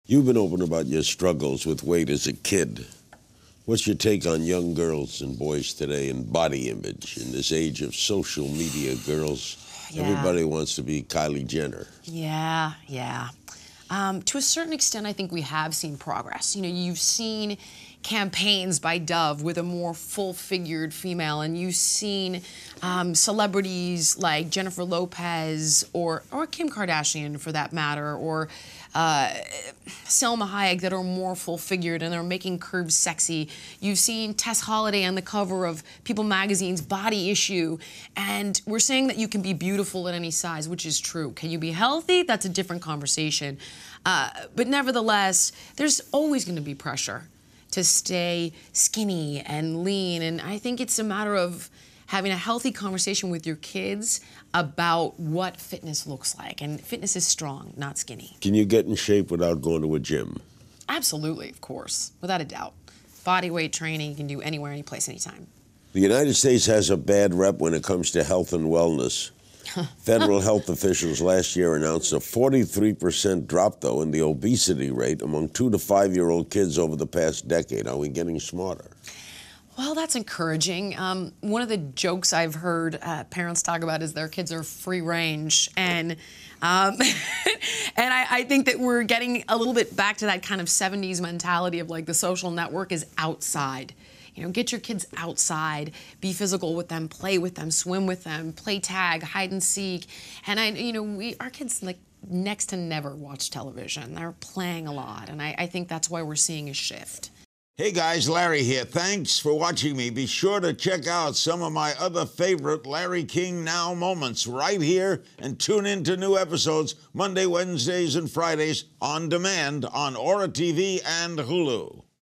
Jillian Michaels On Full Figured Celebs, Body Image and How To Keep Kids Healthy
Health and fitness icon Jillian Michaels praises stars like Jennifer Lopez and Kim Kardashian for celebrating their curves but warns that while "you can be beautiful at any size" you can't necessarily be healthy at any size. Plus, her advice for keeping kids fit and active.